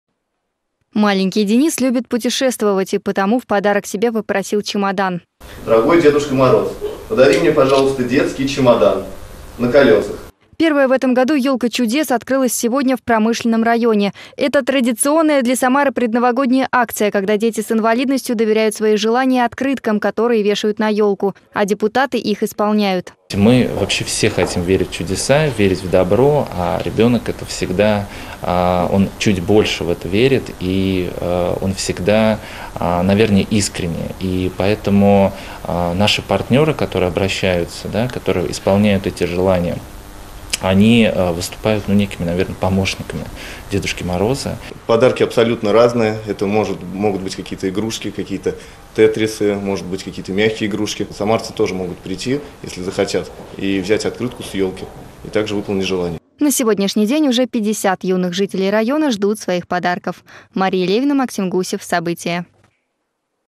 В Самаре стартовала традиционная предновогодняя акция "Ёлка чудес"
В Самаре стартовала традиционная предновогодняя акция "Ёлка чудес". Первые 50 детских желаний уже украсили зелёную красавицу в Промышленном районе. Исполнять их будут депутаты, но к акции могут присоединиться все неравнодушные самарцы. Подробнее в сюжете.